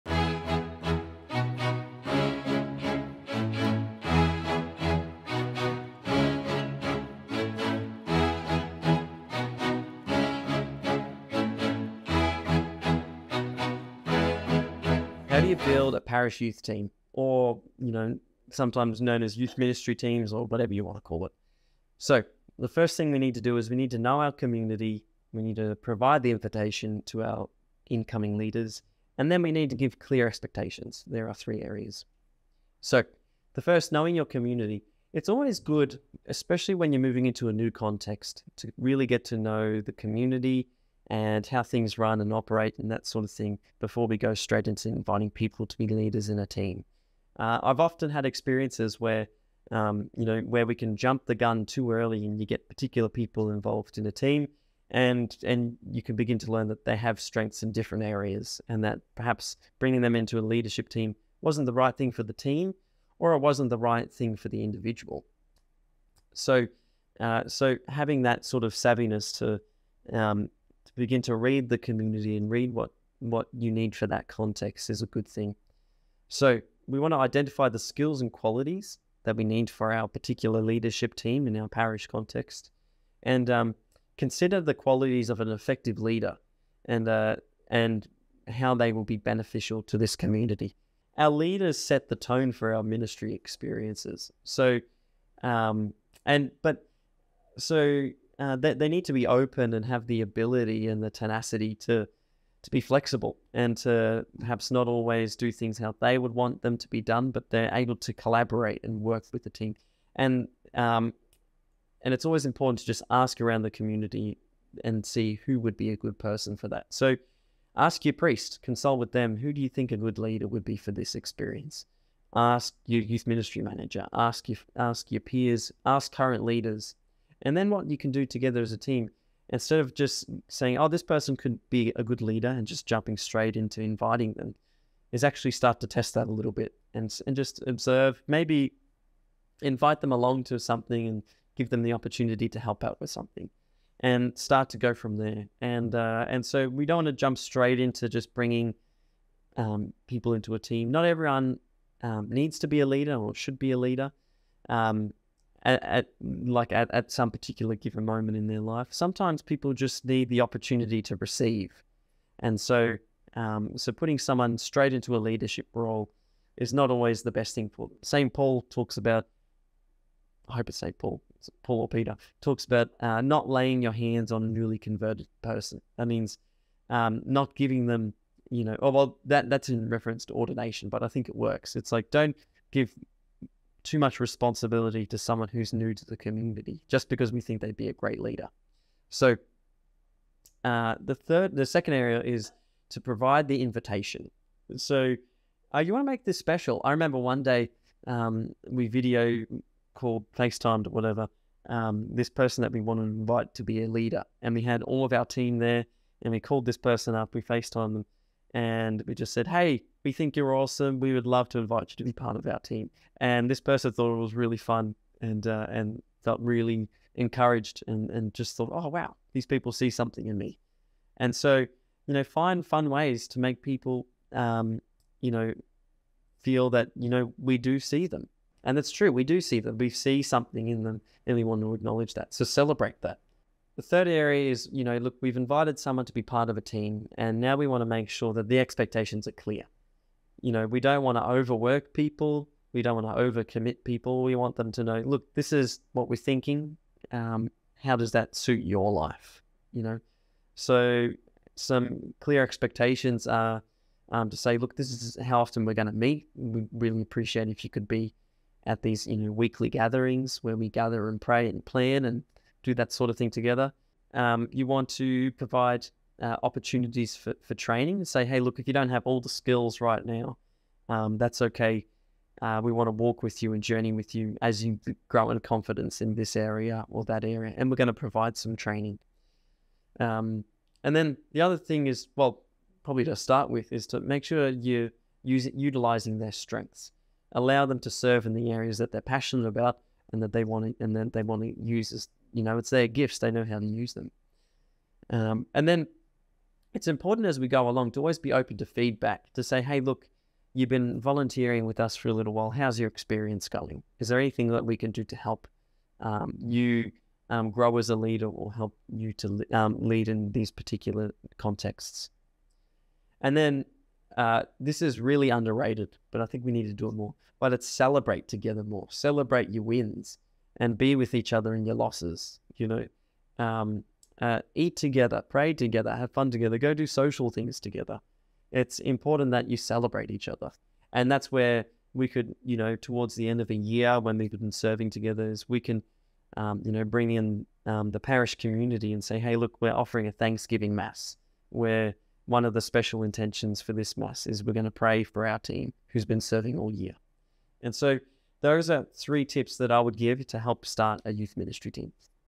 How to Build a Parish Youth Team
Identify leaders in your community, drawing them into a ministry vision, exemplifying their skills and providing them with clear objectives and expectations.